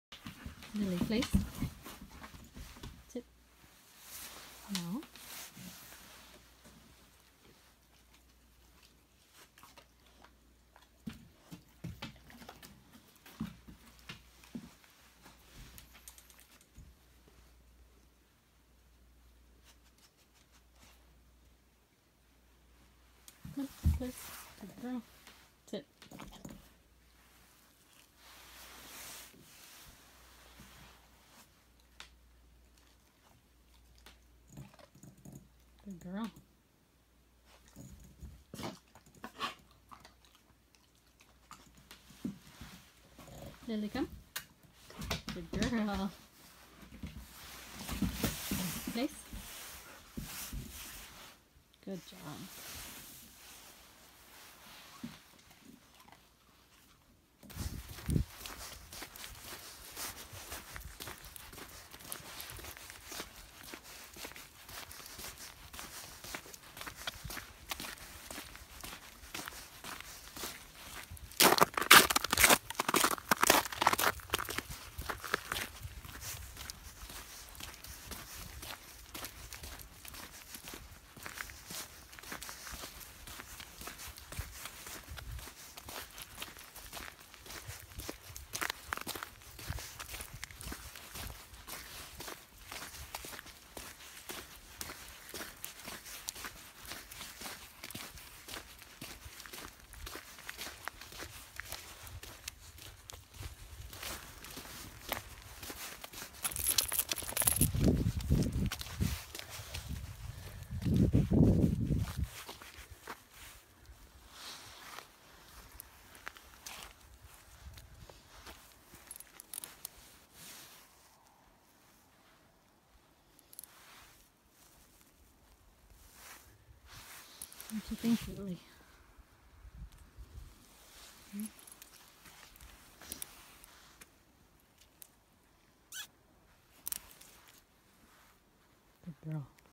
Lily week 1
Working on confidence building and manners.  She has made some big improvements.